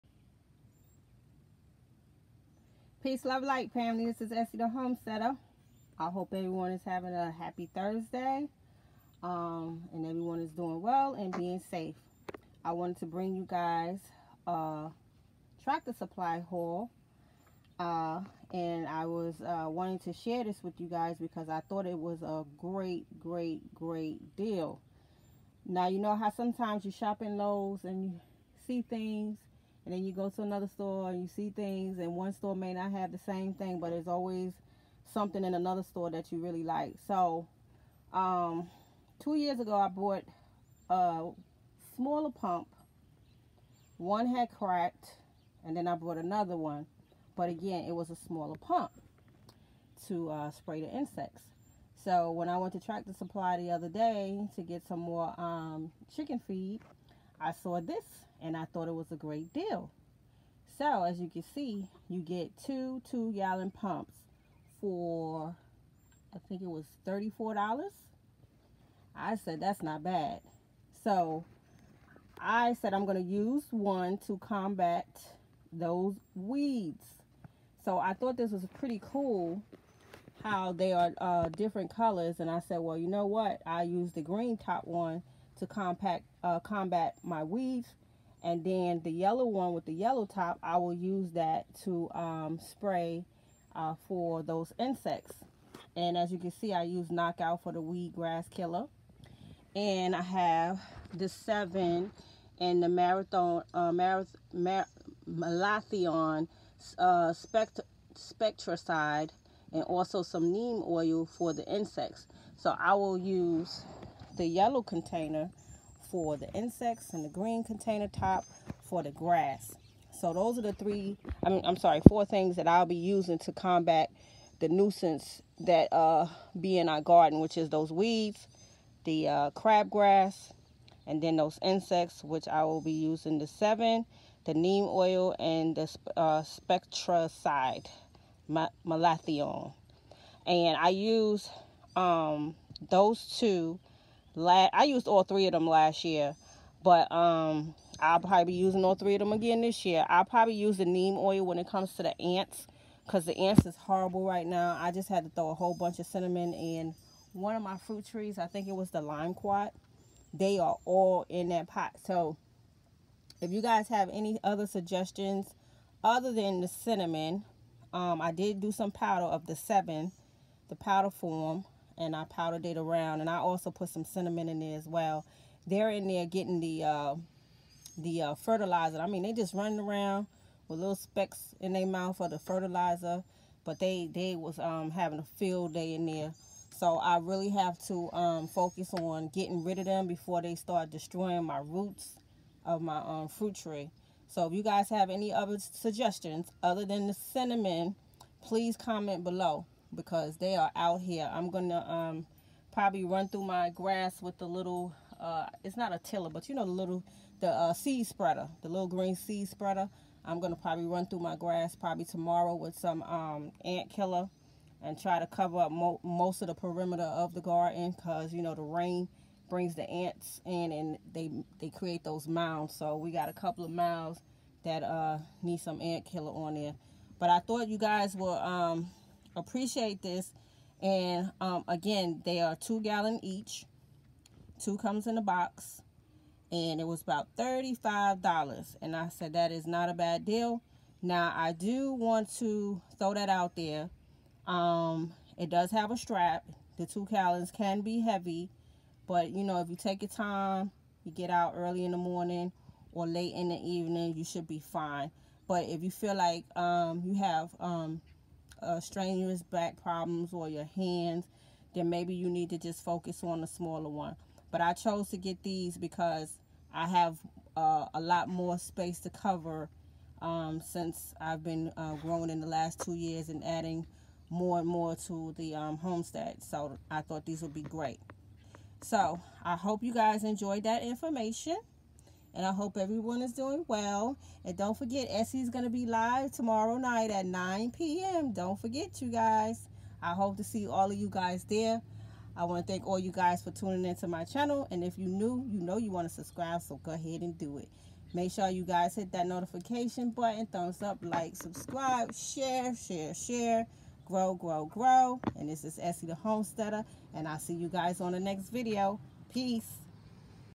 My Tractor supply Haul!
Peace Family! Hope everyone is doing well! So I wanted to share this deal with you guys! This is how I will be handling the nuisances I’m my yard!